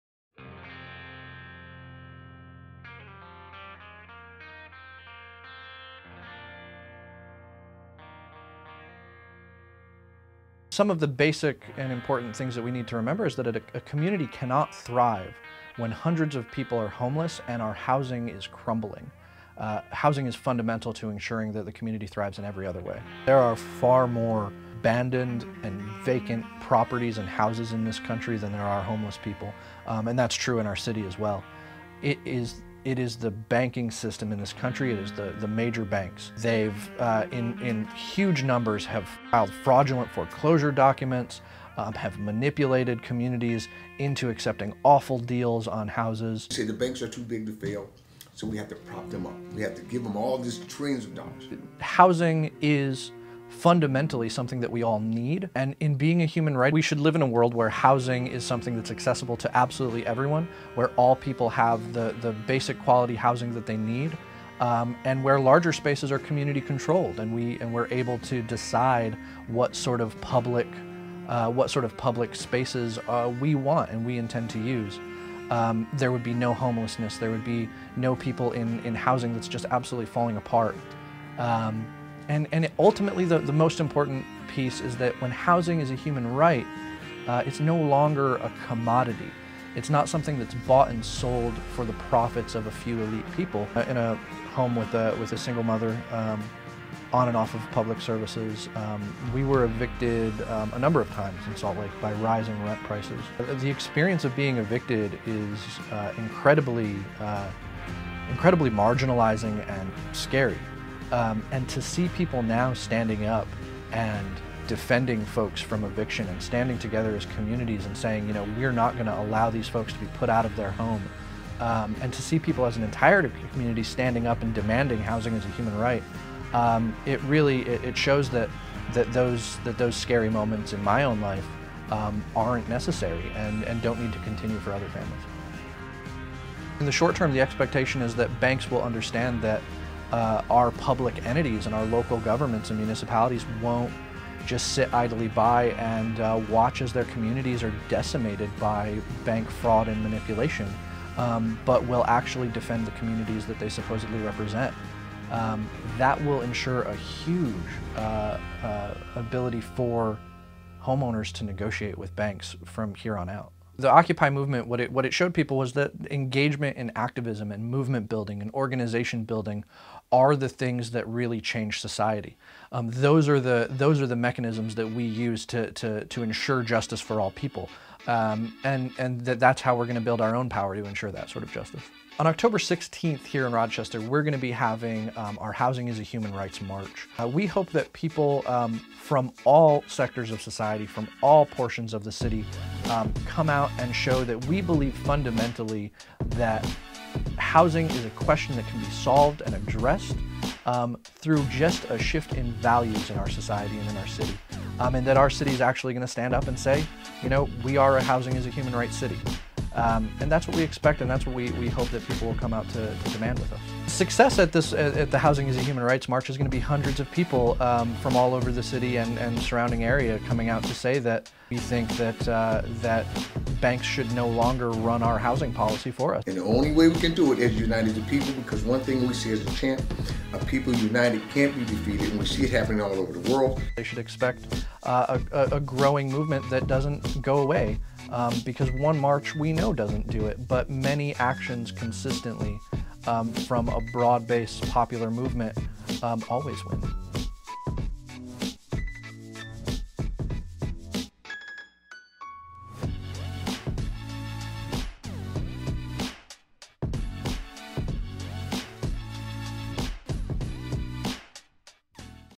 Rochester Housing as a Human Right March
October 16th, activist and organizers in Rochester, NY are coming together to tell the city that we believe housing should be a human right.  Here is a look at some of the issues at play here, how these issues of housing as poverty affect our lives, and how we can build a movement.  Lets see everyone in Washington Square Park on the 16th to set out on a march and rally that will show the people at the top we mean business!